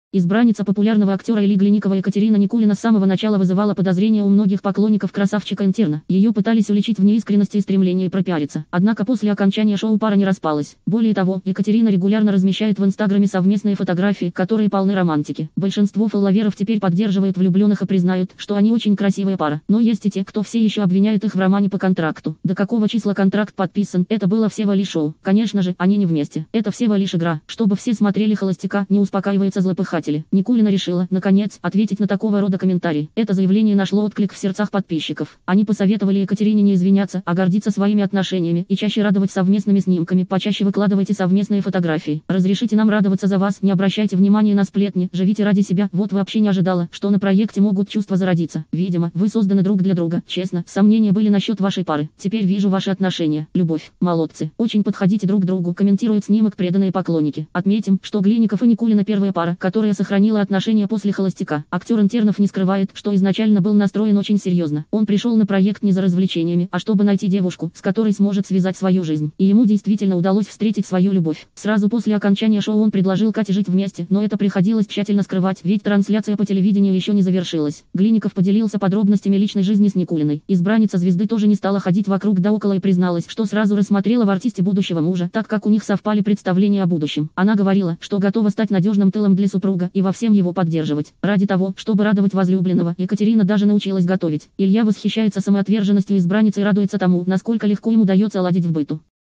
Невеста Ильи Глинникова осадила обвинявших ее в «романе по контракту»
Избранница популярного актера Ильи Глинникова Екатерина Никулина с самого начала вызывала подозрения у многих поклонников красавчика-«интерна». Ее пытались уличить в неискренности и стремлении «пропиариться». Однако после окончания шоу пара не распалась. Более того, Екатерина регулярно размещает в «Инстаграме» совместные фотографии, которые полны романтики.